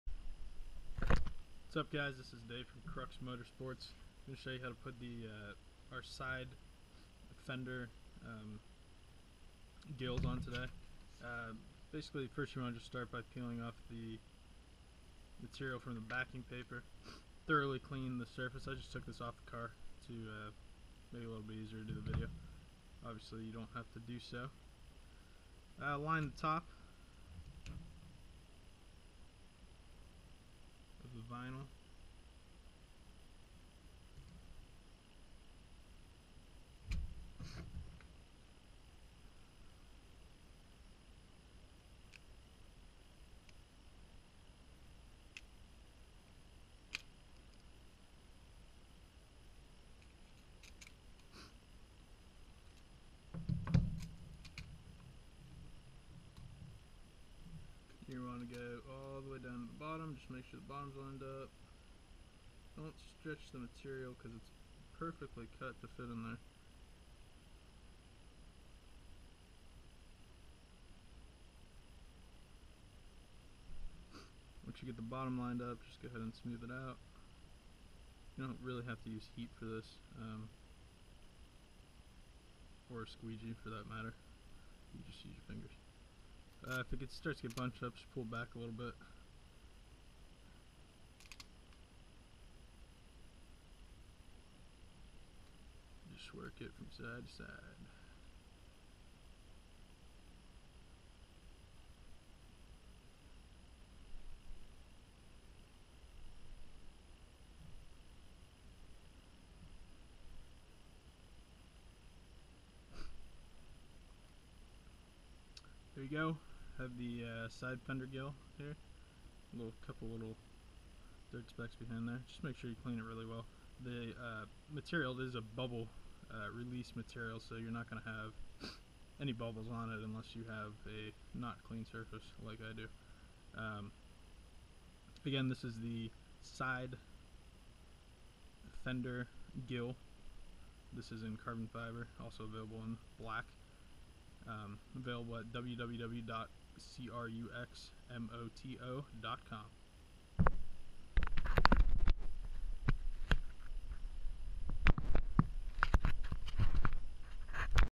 2011 2012 2013 2014 WRX Front Fender Gill Overlay Install
Here we show how to install our precut fender gill overlays for the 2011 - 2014 WRX or STi